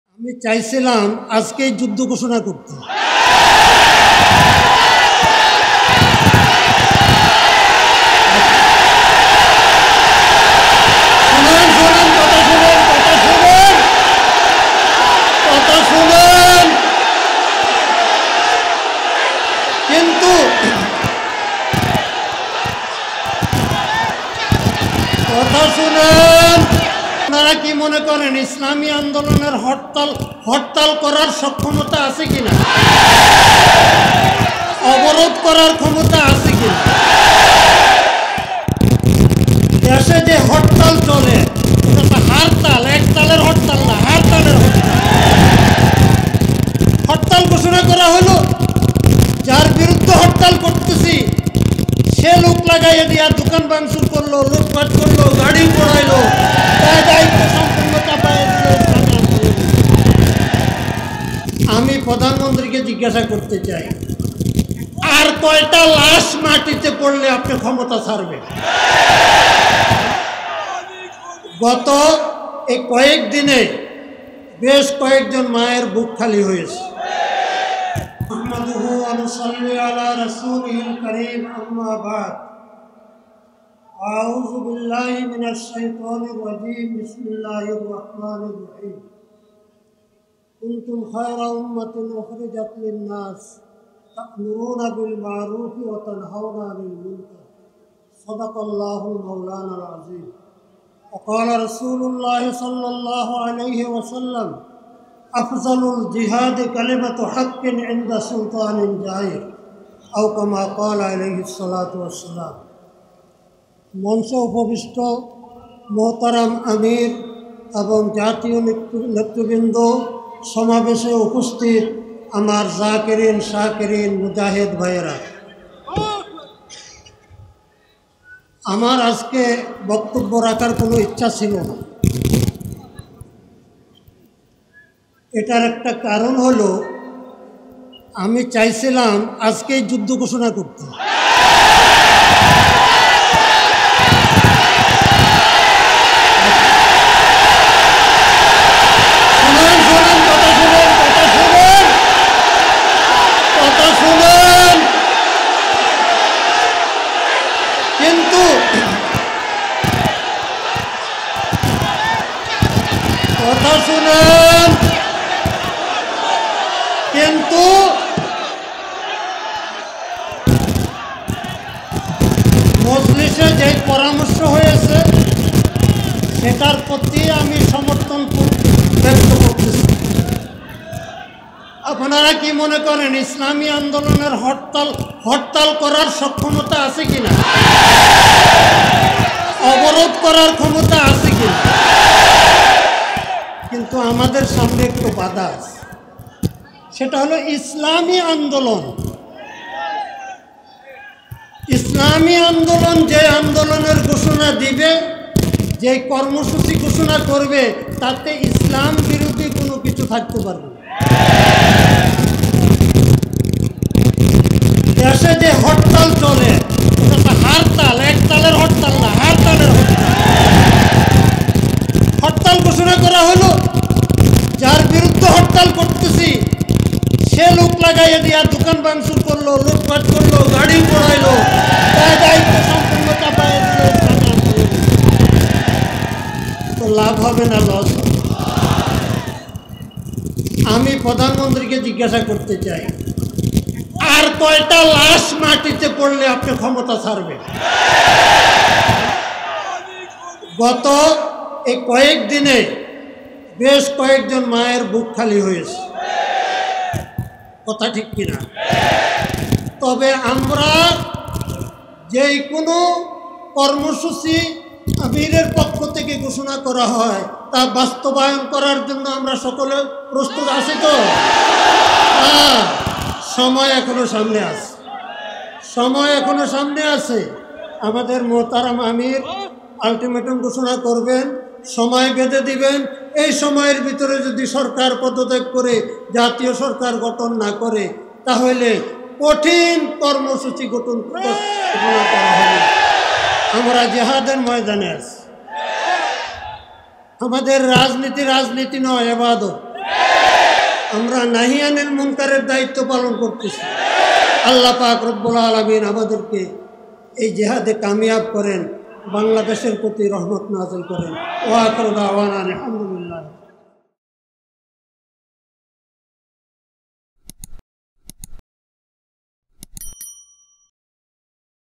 আমি চাইছিলাম আজকেই যুদ্ধ ঘোষণা করতে খালেদ সাইফুল্লাহ | ইসলামি আন্দোলন | Islami Andolon | BNP | News
আমি চাইছিলাম আজকেই যুদ্ধ ঘোষণা করতে খালেদ সাইফুল্লাহ | ইসলামি আন্দোলন | Islami Andolon | National Government | BNP | JAPA | ইসলামী আন্দোলন বাংলাদেশ-এর মহাসমাবেশ

৩ নভেম্বর ২০২৩
ইসলামী আন্দোলন বাংলাদেশ-এর মহাসমাবেশ

ঐতিহাসিক সোহরাওয়ার্দী উদ্যানে বক্তব্য দিলেন
আল্লামা খালেদ সাইফুল্লাহ
Allama Khaled Saifullah